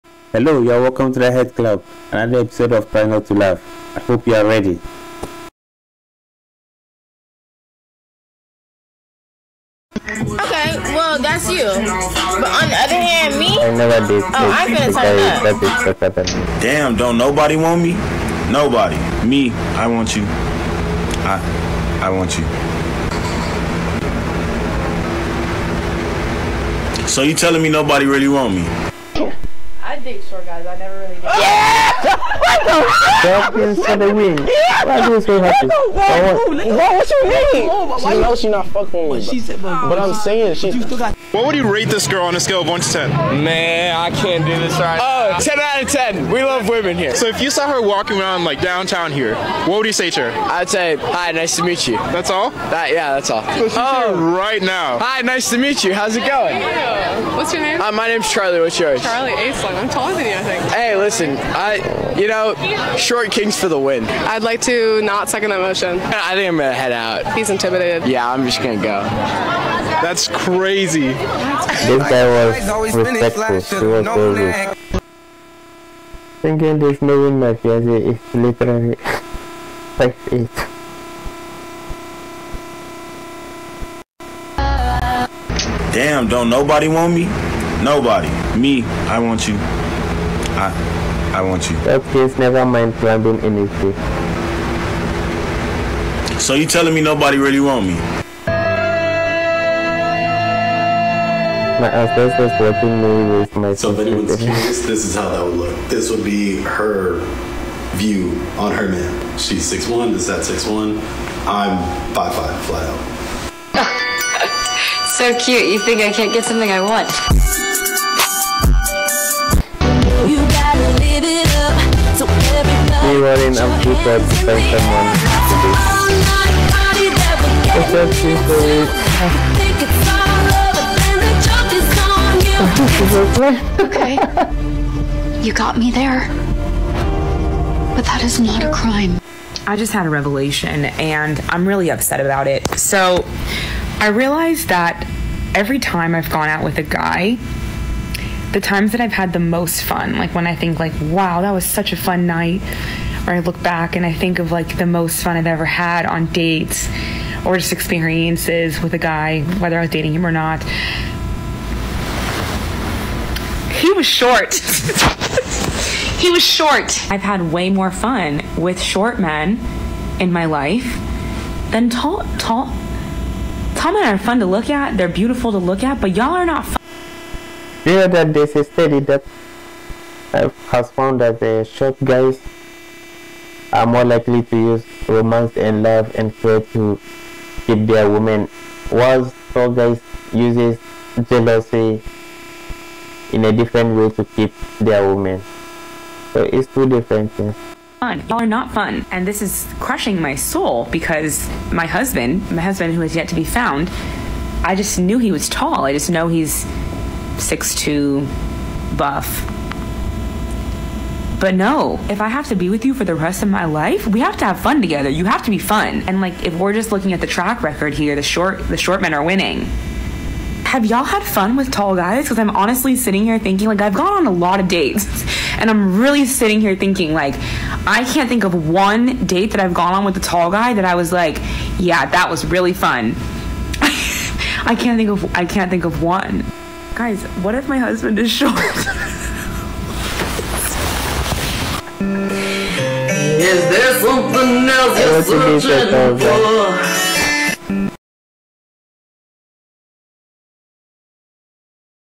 When She is Taller than You 😍 | Tall women and Short Guys
Tall and short funny video from tiktok that will crack you up with laugh